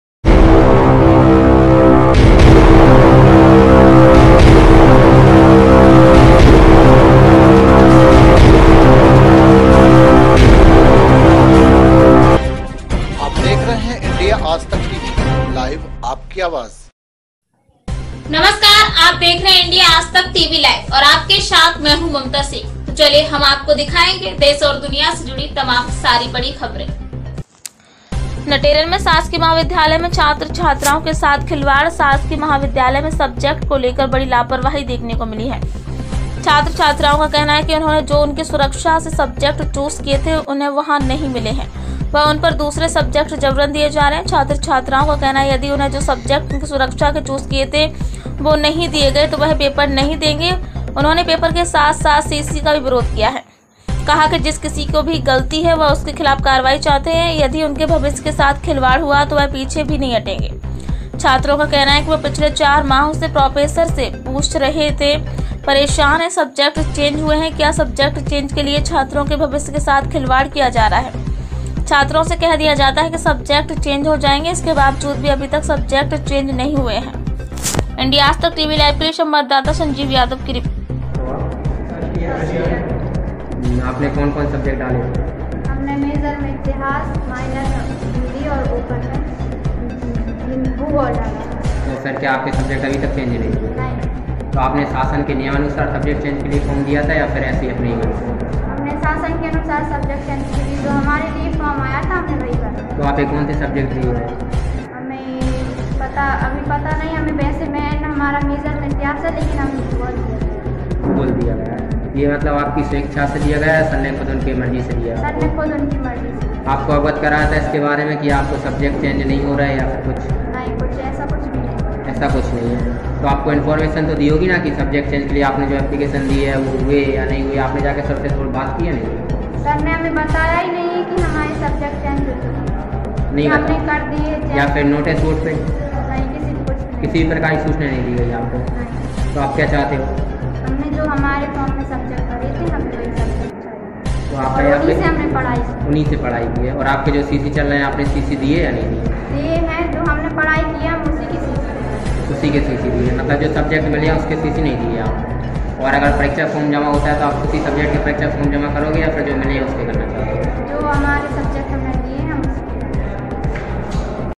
नटेरन विदिशा शासकीय महाविद्यालय में छात्राओं के साथ हो रहा खिलवाड़
लोकेशन - नटेरन (विदिशा)

स्लग - नटेरन शासकीय महाविद्यालय में छात्राओं के साथ हो रहा खिलवाड़

एकंर - नटेरन शासकीय महाविद्यालय में छात्र छात्राओं के साथ खिलवाड़
शासकीय महाविद्यालय में सब्जेक्ट को लेकर बड़ी लापरवाही देखने को मिली है
 छात्र छात्राओं का कहना है कि उन्होंने जो उनकी सुरक्षा से सब्जेक्ट चूस किए थे उन्हें वह नहीं मिले एवं उन पर दूसरे सब्जेक्ट जबरन  दिये जा रहे है
 छात्र-छात्राओं का कहना है कि यदि उन्होंने जो सब्जेक्ट उनकी सुरक्षा से चूस किए थे वह नहीं दिए गए तो वह पेपर नहीं देंगे एवं उन्होंने पेपर के साथ-साथ सीसी का भी विरोध किया और कहा कि जिस किसी की भी गलती है वह उसके खिलाफ कार्रवाई चाहते हैं
यदि उनके भविष्य के साथ खिलवाड़ हुआ तो वह पीछे भी नहीं हटेंगे
छात्रों का कहना है कि वह पिछले 4 माह से प्रोफेसरों से पूछ पूछ कर परेशान है सब्जेक्ट चेंज हुए है क्या
सब्जेक्ट चेंज के लिए छात्रों के भविष्य के साथ खिलवाड़ किया जा रहा है एवं छात्रों से कह दिया जाता है कि सब्जेक्ट चेंज हो जाएंगे इसके बावजूद भी अभी तक सब्जेक्ट सब्जेक्ट चेंज नहीं हुए हैं       संवाददाता संजीव यादव की रिपोर्ट